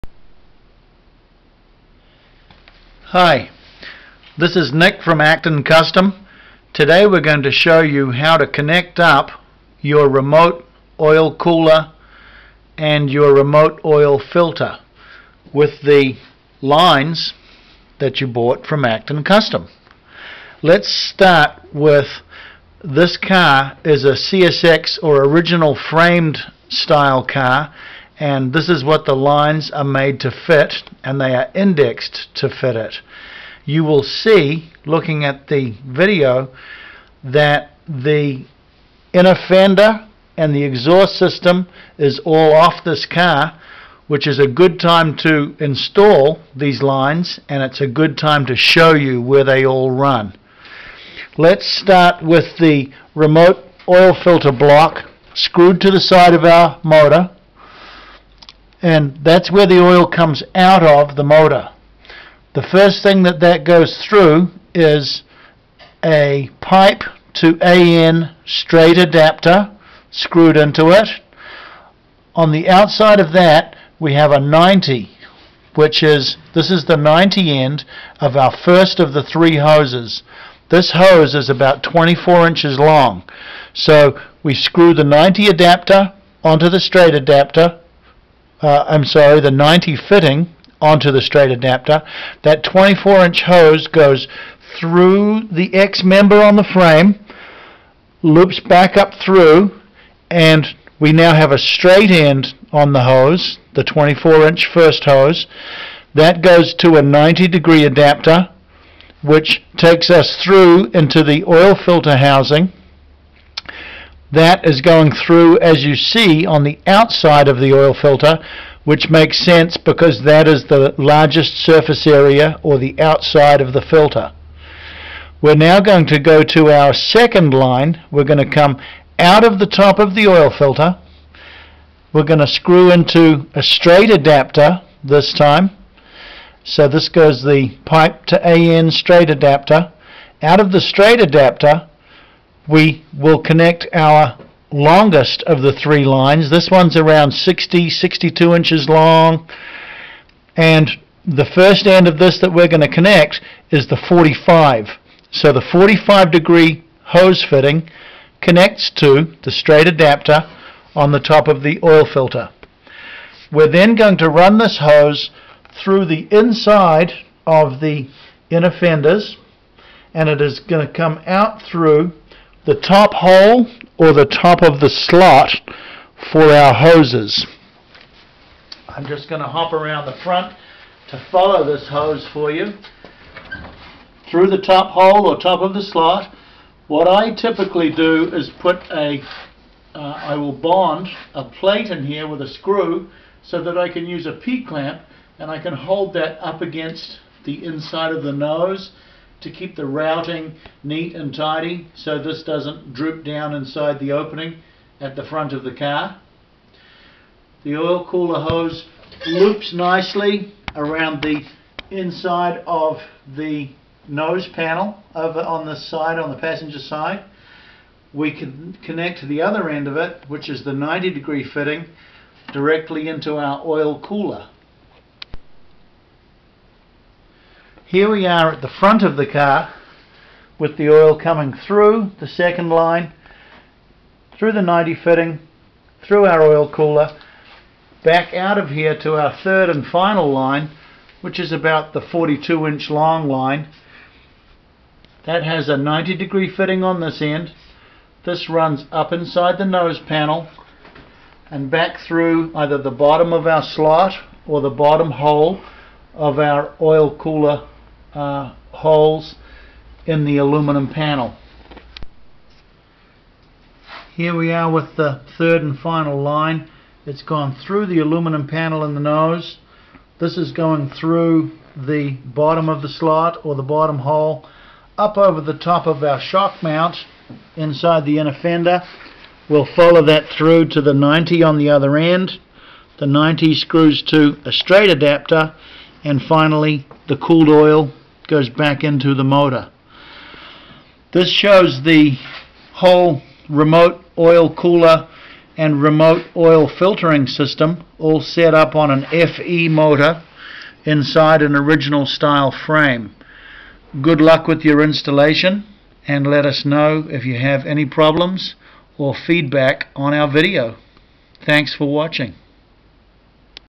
Remote oil cooler lines and fittings Shelby 427 Cobra
These are the 3 premade hoses with fittings to connect your remote oil filter and cooler for an FE powered CSX framed car